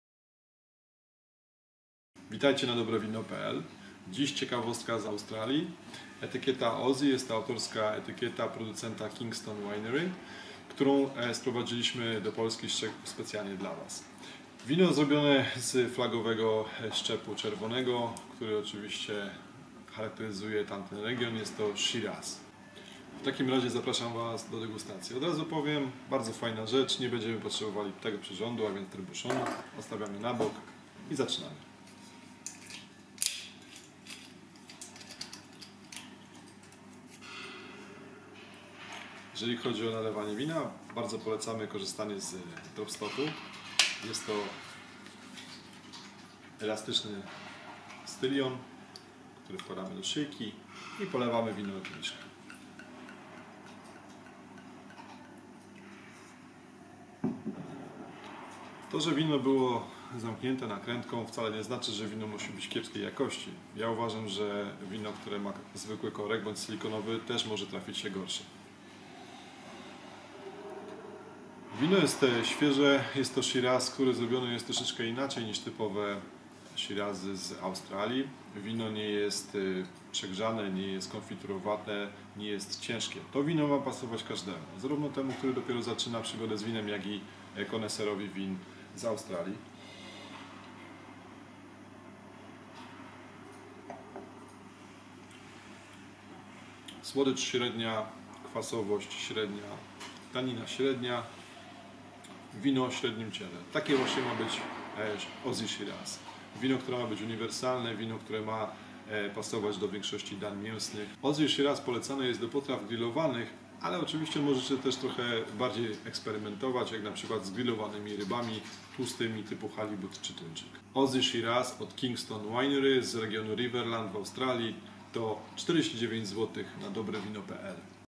Ozzi Shiraz - czerwone wino australijskie
Przed Wami klasyczna, australijska propozycja prosto z regionu Riverland.
Stworzone w 100 % z owoców szczepu Shiraz, jednego z najbardziej popularnych w Australii. Ma rubinową barwę z purpurowymi refleksami i intensywny aromat dojrzałych porzeczek, malin i truskawek z delikatną nutą dębu. W smaku wyraźnie wyczuwalne są jagody, jeżyny i słodkie maliny. Delikatne taniny i aksamitna konsystencja pieszczą podniebienie zarówno tych, którzy mają za sobą mnóstwo winnych doświadczeń, jak i tych, którzy dopiero zaczynają swoją przygodę.

Poznajcie wino OZZI SHIRAZ